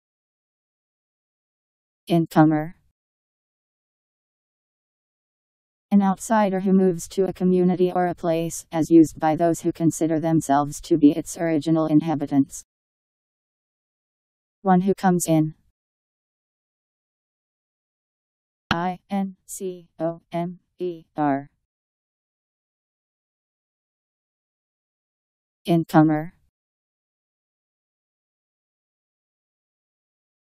Incomer Meaning
Video shows what incomer means. An outsider who moves to a community or a place, as used by those who consider themselves to be its original inhabitants.. One who comes in..  Incomer Meaning. How to pronounce, definition audio dictionary. How to say incomer. Powered by MaryTTS, Wiktionary